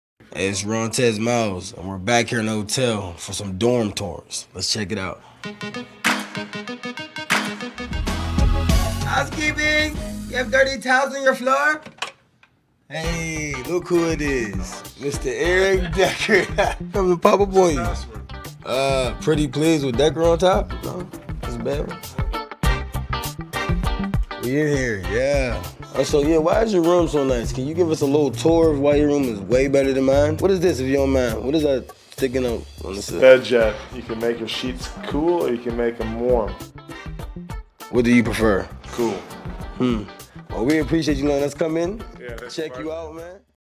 Eric Decker using his BedJet at NY Jets Training Camp!
Eric Decker shares his BedJet during a training camp dorm tour with Rontez Miles.